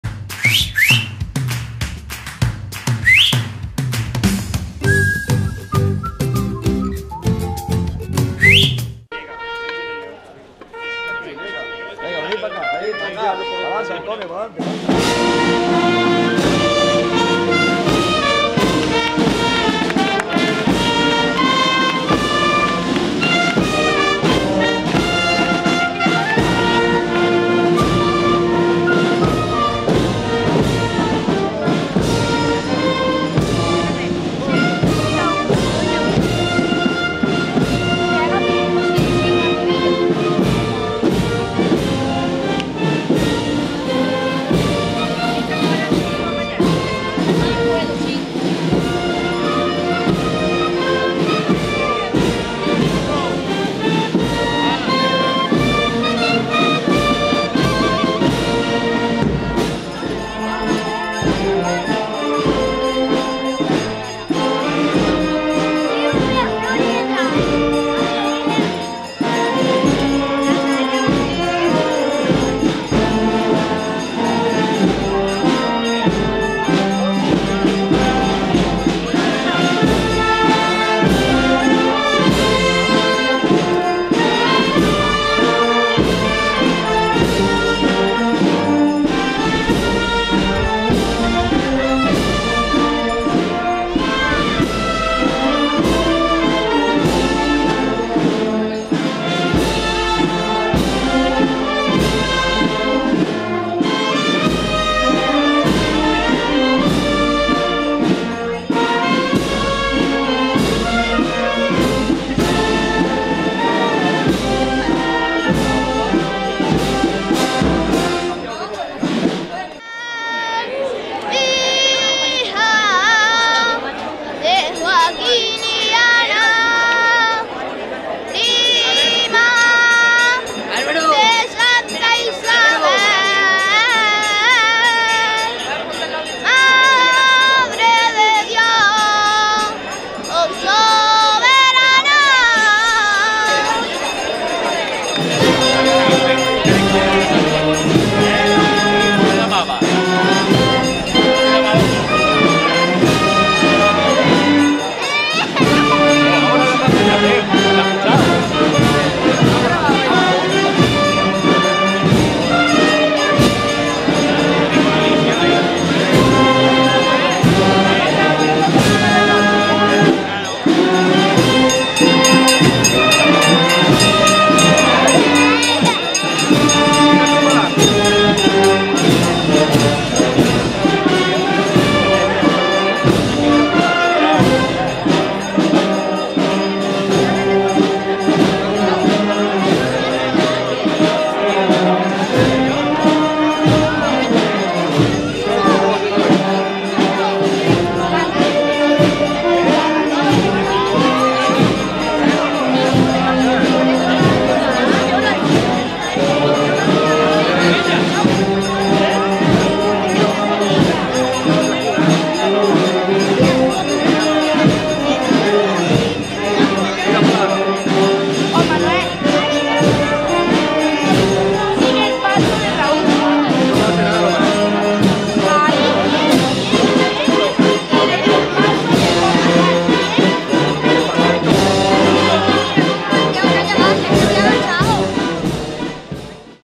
SÁBADO DE RAMOS | Semana Santa Chiquita Puente Genil
Salida procesional de la Virgen de la Guía del año 2019 correspondiente al Sábado de Ramos de la Semana Santa Chiquita de Puente Genil.